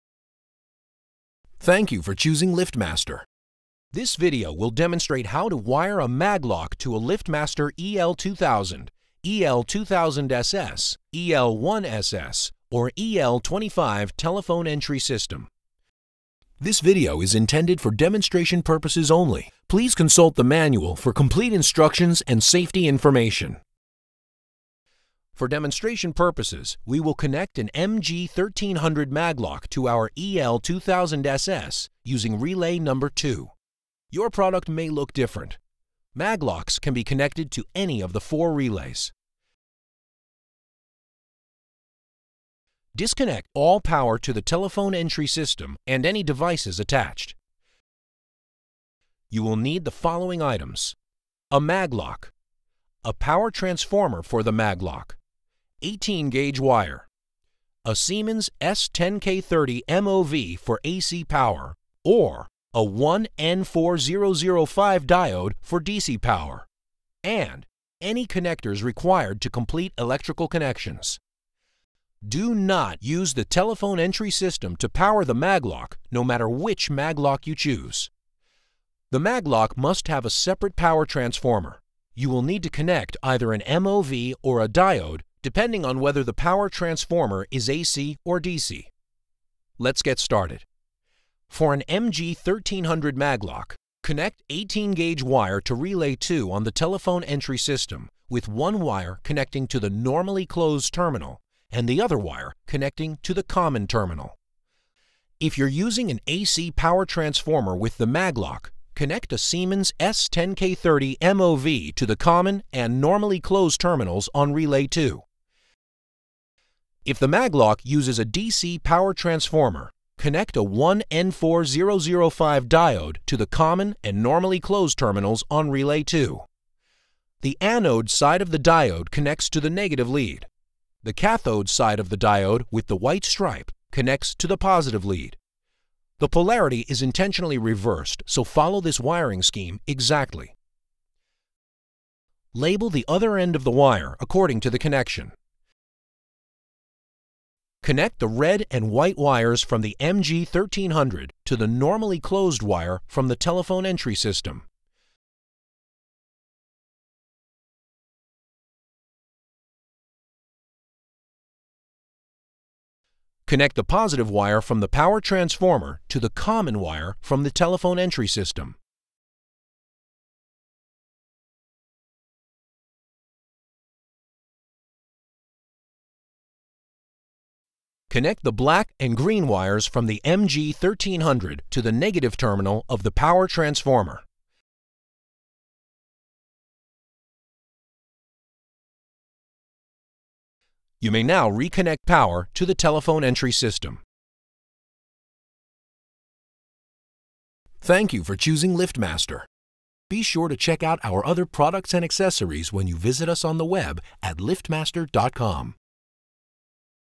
LiftMaster - How to Wire a Maglock to the EL2000, EL2000SS EL1SS or EL25 Telephone Entry System
Wiring a gate maglock to a LiftMaster telephone entry system takes just minutes. Make sure to always use a separate power supply for your maglock.